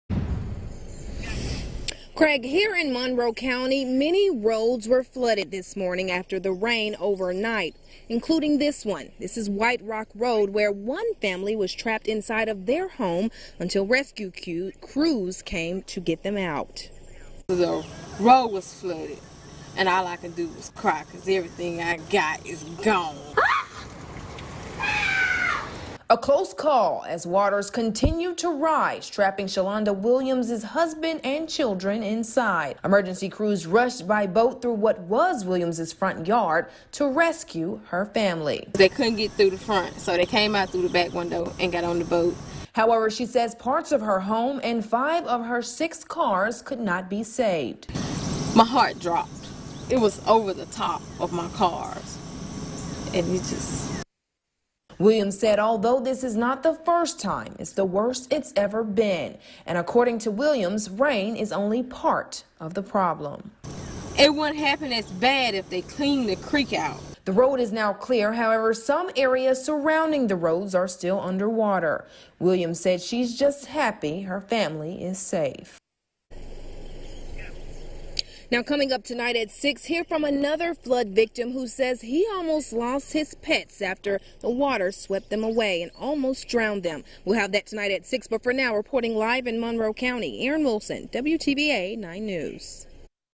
Family trapped in home by flood waters
Family trapped in home by flood waters after sever storms swept through north Mississippi.